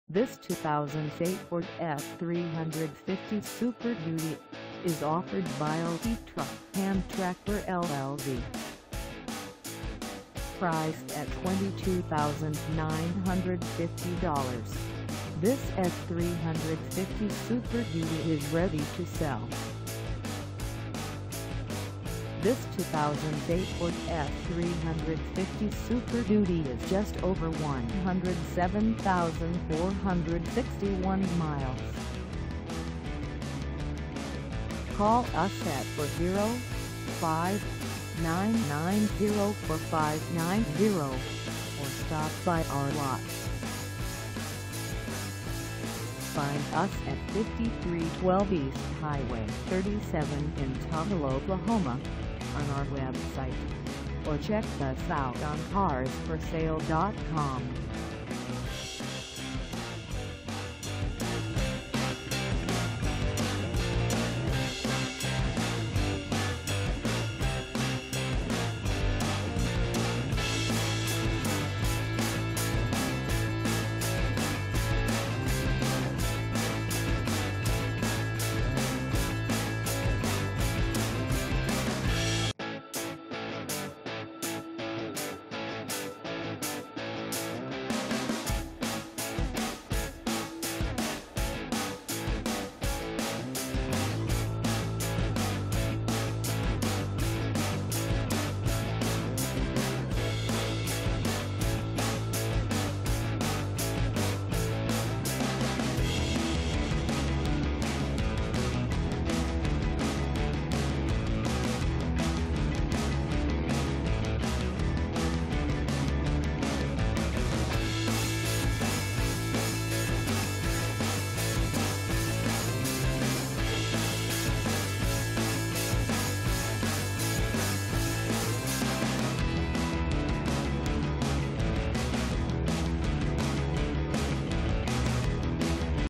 2008 Ford F-350 Super Duty XL 4dr SuperCab for sale in Tuttl
This 2008 Ford F-350 Super Duty XL 4dr SuperCab is for sale in Tuttle, OK 73089 at OT Truck and Tractor LLC.

Engine: V8 6.4L Twin Turbocharger
Transmission: Automatic 6-Speed
Color: White
Mileage: 107461
Contact OT Truck and Tractor LLC 5312 E Highway 37 Tuttle, OK 73089 to test drive this 2008 Ford F-350 Super Duty XL 4dr SuperCab today.